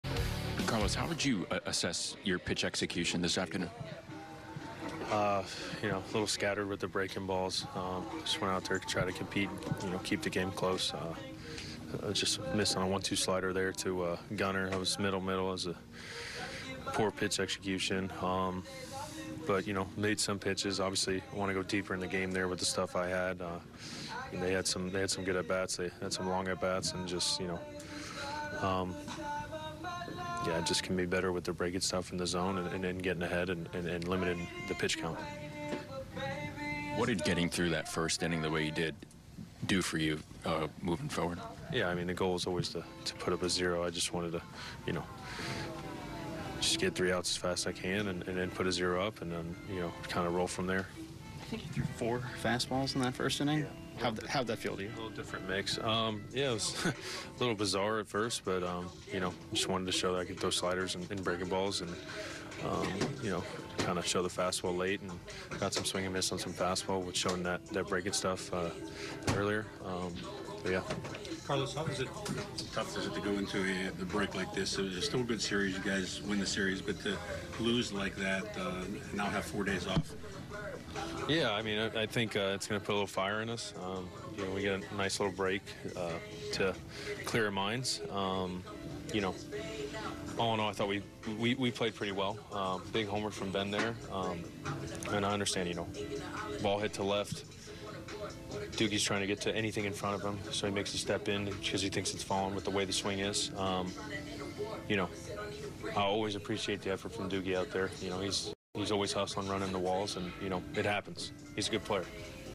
Carlos Rodón on his 4-inning outing in Baltimore
Carlos Rodón gives an assessment on his pitch execution against the Orioles.